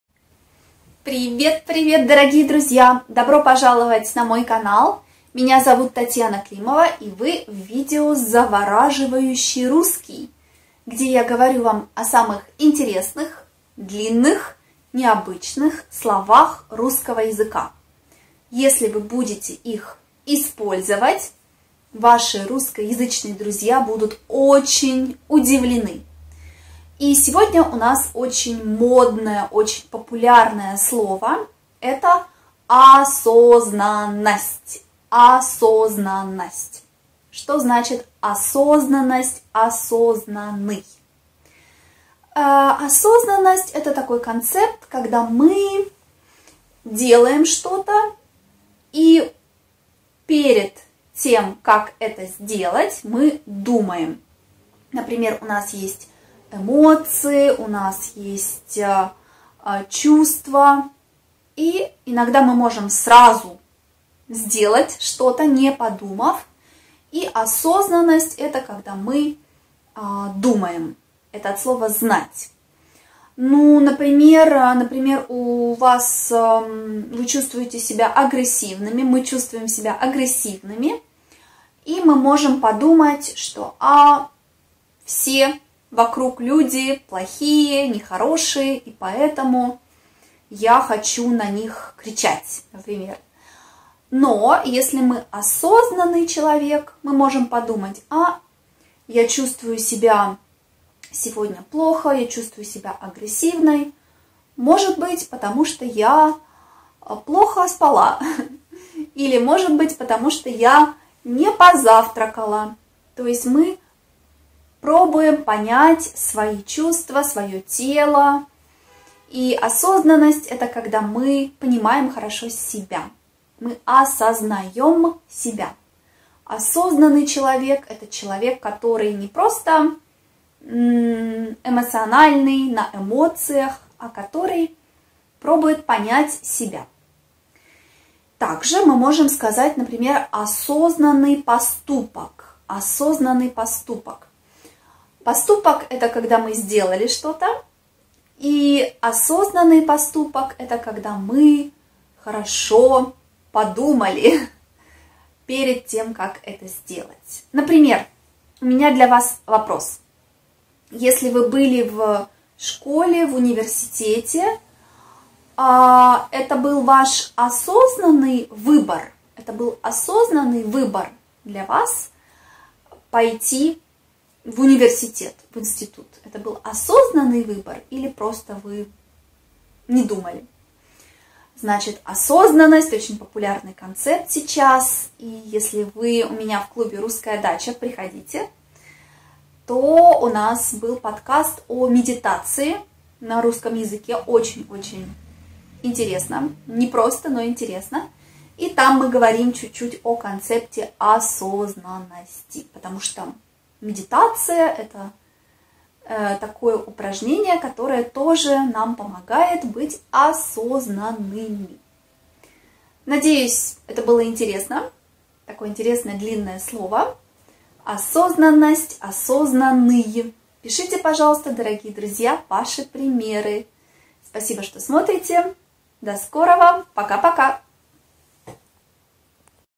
ОСОЗНАННОСТЬ 💫 SLOW RUSSIAN VIDEO
Сегодня у нас слово "Осознанность". Что это значит? Что значит "осознанный человек"?

Вы в Клубе Русская Дача? Тогда слушайте подкаст-медитацию на русском языке. Осознанность, внимание, дыхание и другие важные слова. Уровень: В2

SLOW RUSSIAN video.